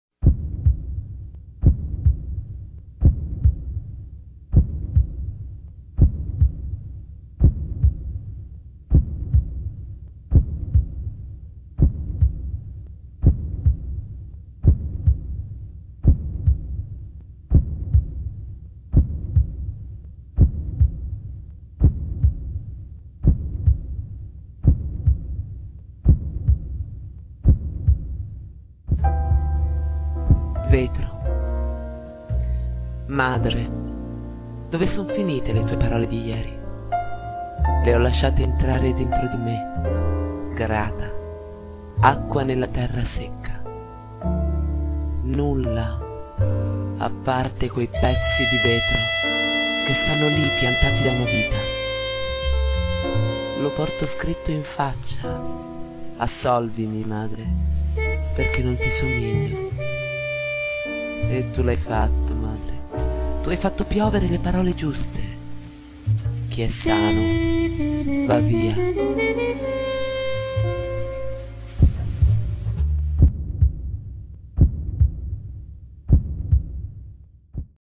"Vetro" di Bianca Madeccia
Titolo: "Vetro", testo di Bianca Madeccia. Voce recitante: Rossana Carturan. Video a cura di Bianca Madeccia